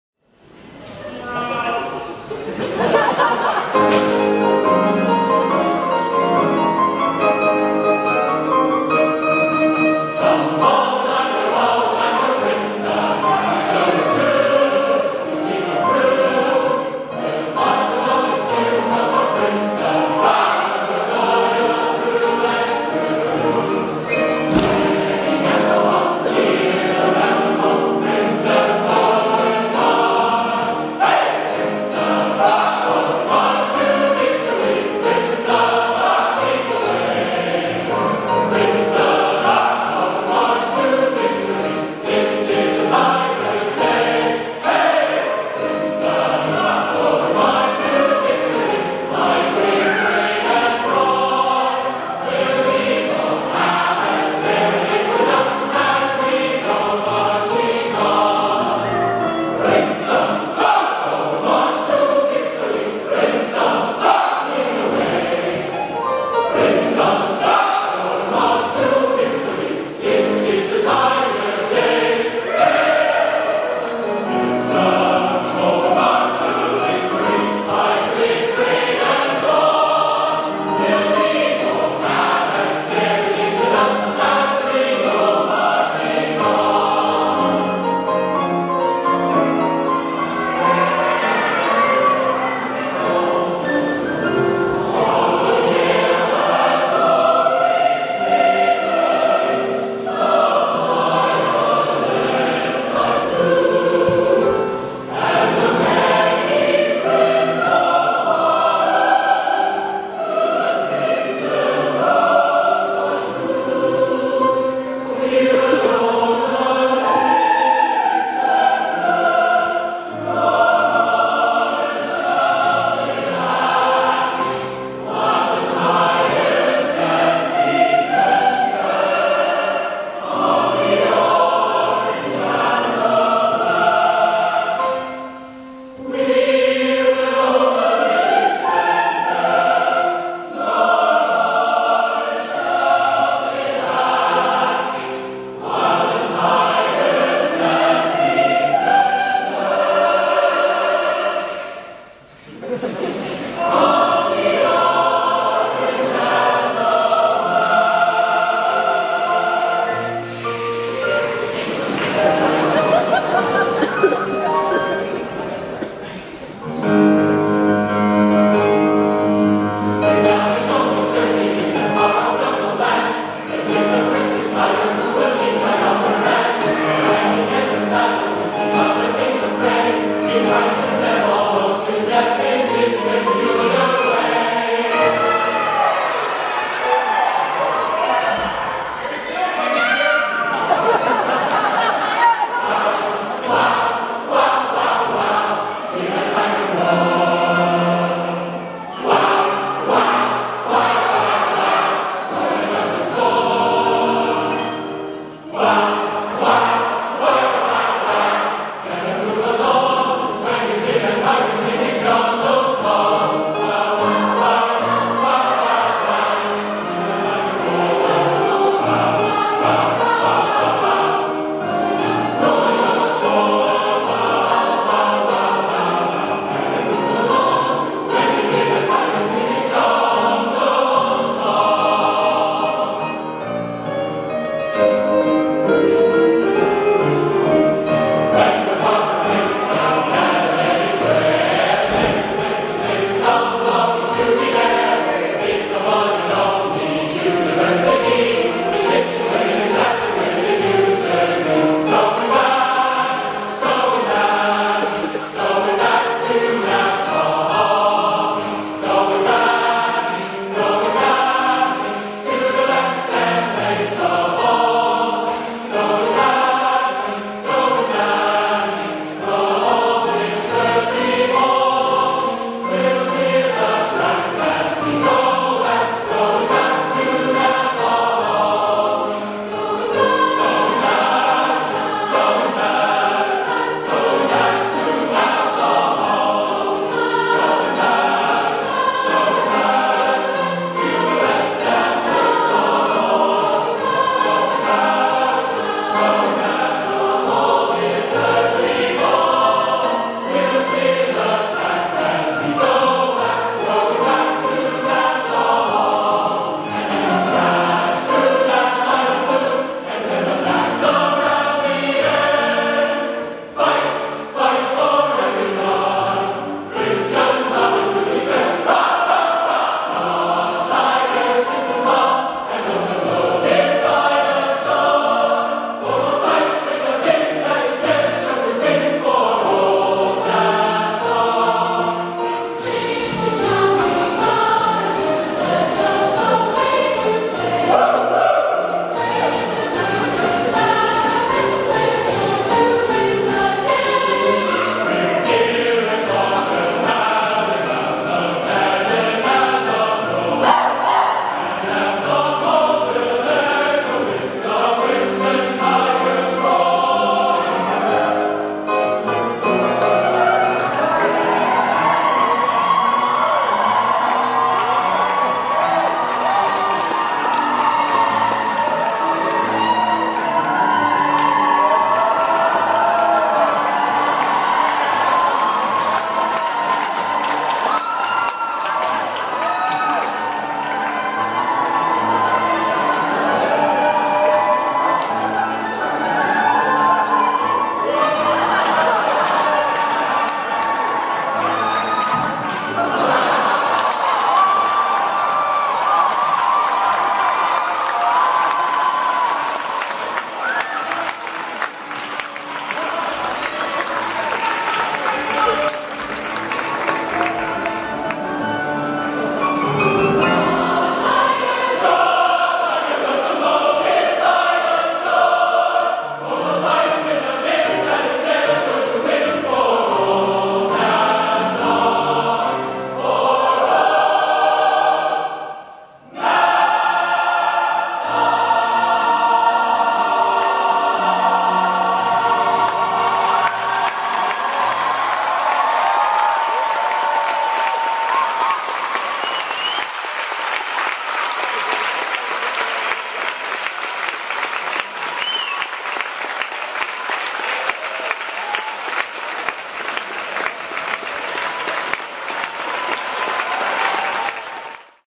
Princeton-Harvard Football Concert: Football Medley 2014
October 2014, Glee Club Football Concert Centennial

Conducted by James Walsh '15
Piano: Doris Lee '14 and Kevin Lee '16